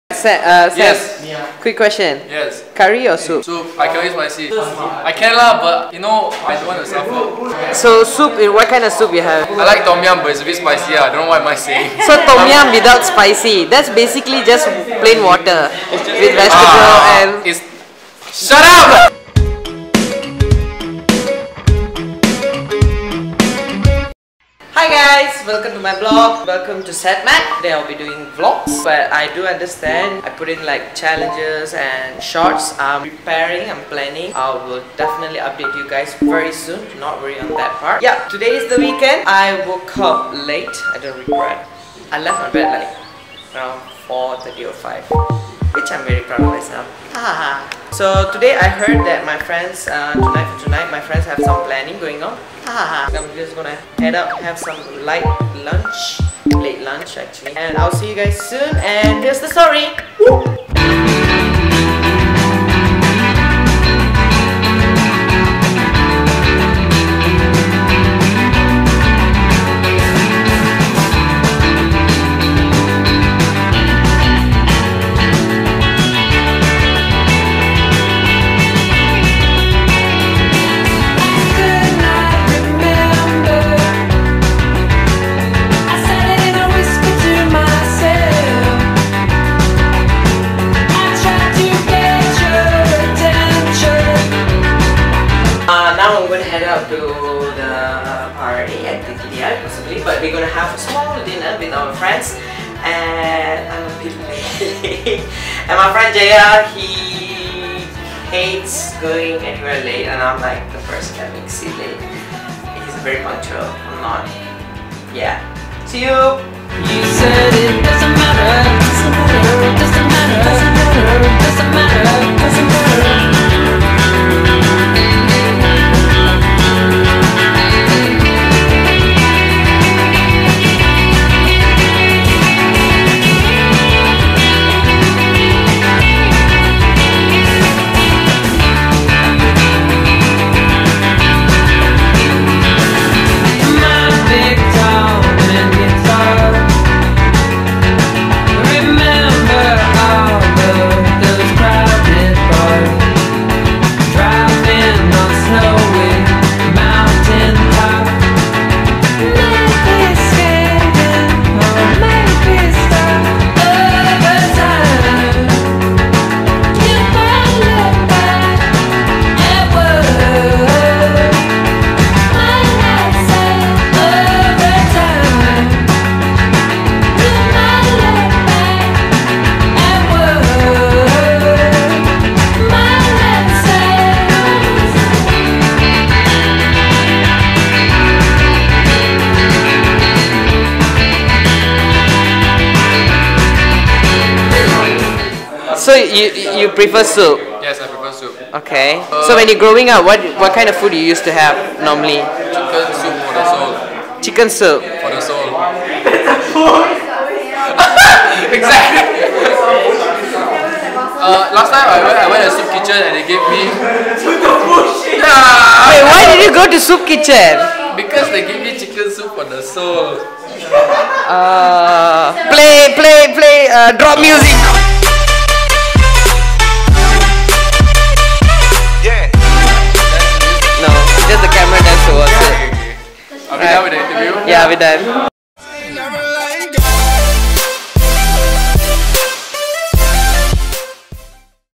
Vlog #004 = Soul Chicken?
This Vlog I'll be talking about my birthday party and a short interview with Samuel from Iact Christian Fellowship.

Do catch me over my other Social Net. Accnts